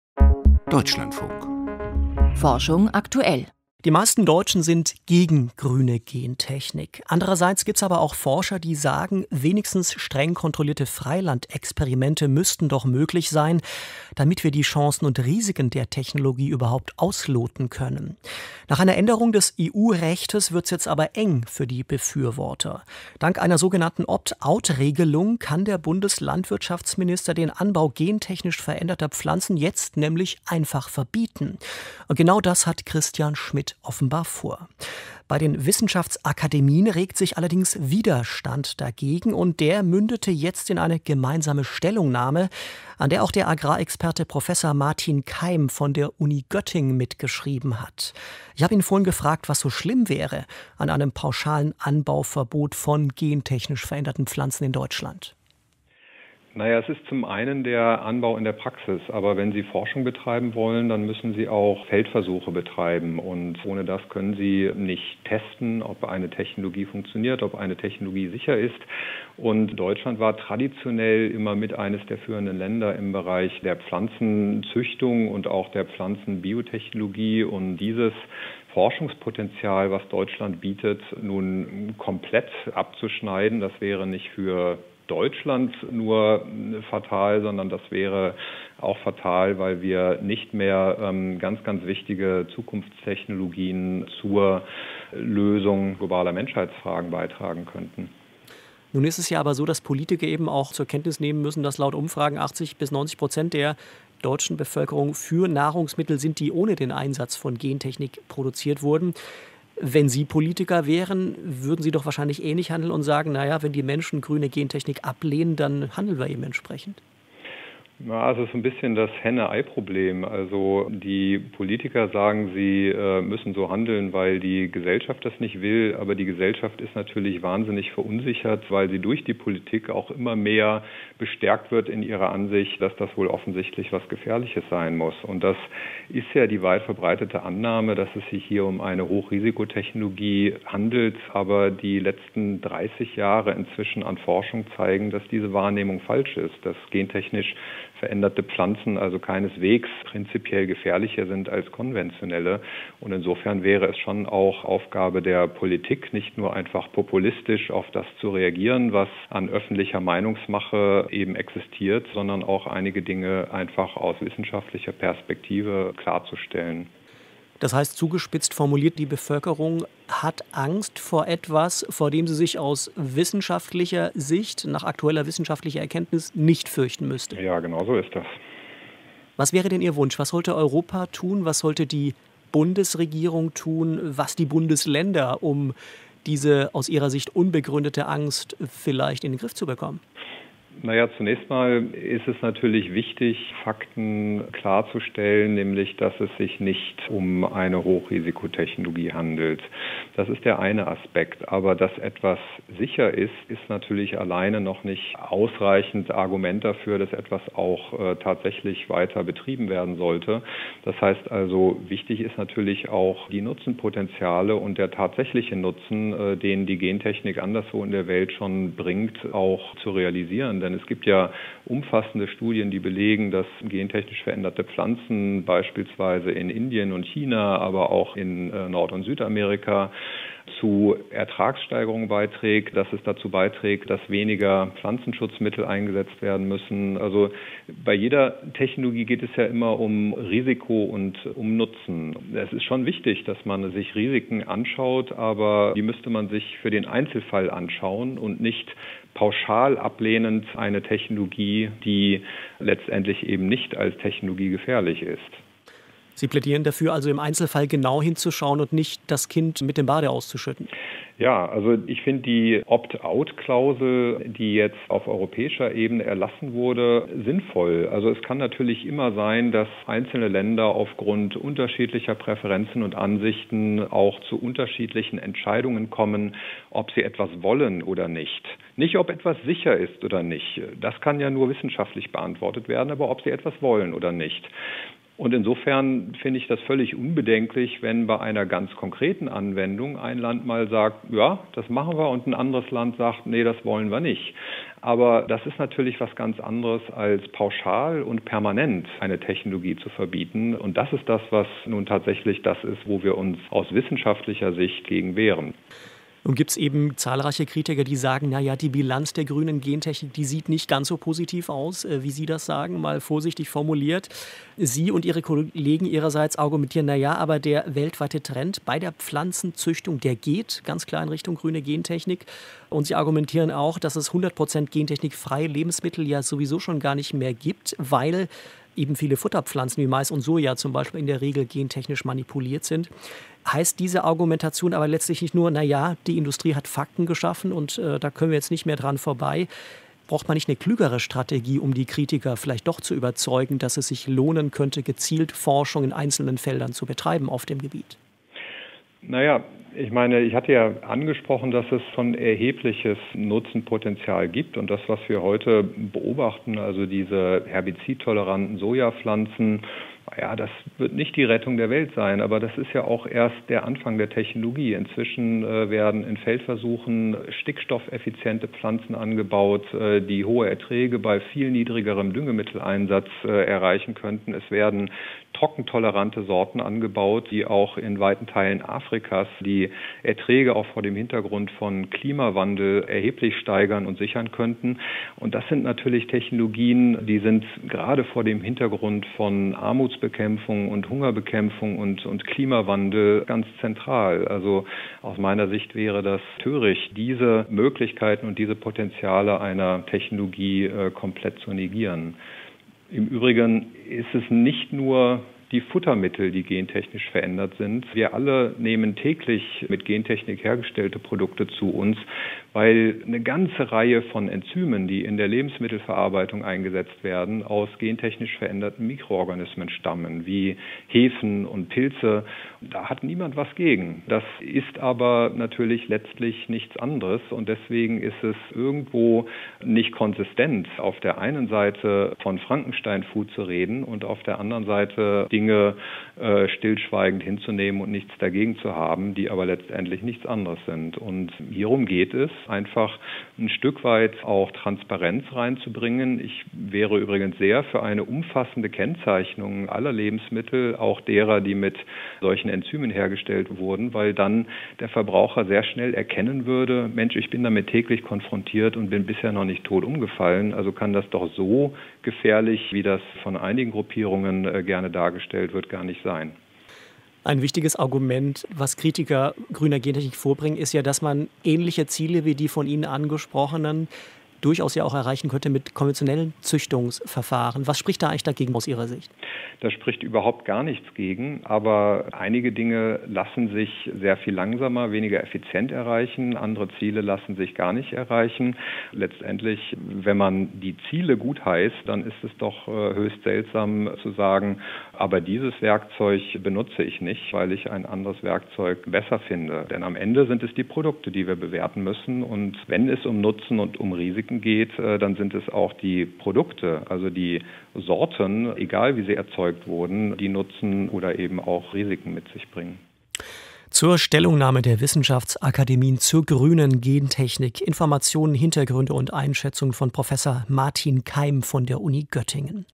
Grüne Gentechnik ist keine Hochrisikotechnologie!
Es gebe umfassende Studien, die belegen, dass gentechnisch veränderte Pflanzen mitunter den Einsatz von Pestiziden verringern, sagte Matin Qaim. (picture-alliance / ZB)

Bundeslandwirtschaftsminister Christian Schmidt möchte den Anbau gentechnisch modifizierter Pflanzen verbieten. Dagegen wendet sich Matin Qaim, Professor für Agrarökonomie in Göttingen. Gentechnisch veränderte Pflanzen seien keineswegs prinzipiell gefährlicher als konventionelle, sagte er im DLF.

"Wenn Sie Forschung betreiben wollen, dann müssen Sie auch Feldversuche betreiben", sagte Matin Qaim, Professor für Agrarökonomie an der Uni Göttingen, im Deutschlandfunk. Ohne das könne man nicht testen, ob eine Technologie funktioniert, ob eine Technologie sicher ist.

Bezüglich gentechnisch veränderter Pflanzen sei die Gesellschaft wahnsinnig verunsichert - "weil sie durch die Politik auch immer mehr bestärkt wird in der Ansicht, dass das offensichtlich etwas gefährliches sein muss."

Die weit verbreitete Annahme sei, dass es sich bei grüner Gentechnik um eine Hochrisikotechnologie handle. "Aber die letzten 30 Jahre an Forschung zeigen, dass diese Wahrnehmung falsch ist." Gentechnisch veränderte Pflanzen seien keineswegs prinzipiell gefährlicher als konventionelle. "Insofern wäre es schon auch Aufgabe der Politik, nicht nur einfach populistisch auf das zu reagieren, was an öffentlicher Meinungsmache existiert, sondern einige Dinge einfach aus wissenschaftlicher Perspektive klarzustellen."

Es gebe umfassende Studien, die belegen, dass gentechnisch veränderte Pflanzen - beispielsweise in Indien und China - zur Ertragssteigerung beitragen, sagte Matin Qaim.

Zudem sei Deutschland traditionell mit eines der führenden Länder im Bereich der Pflanzenzüchtungen und auch der Pflanzen-Biotechnologie."Und dieses Forschungspotenzial, was Deutschland bietet, nun komplett abzuschneiden, wäre nicht nur für Deutschland fatal, sondern das wäre auch fatal, weil wir nicht mehr ganz, ganz wichtige Zukunftstechnologien zur Lösung globaler Menschheitsfragen beitragen könnten.